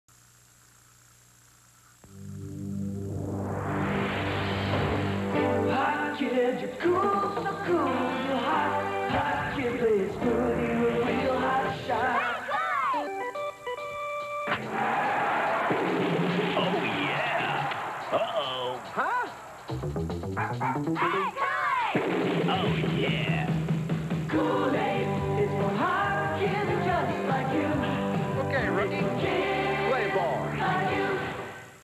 Pete Rose Kool-Aid Commercial (1986)
Television commercial for Kool-Aid featuring a futuristic game of baseball with Pete Rose. (1986)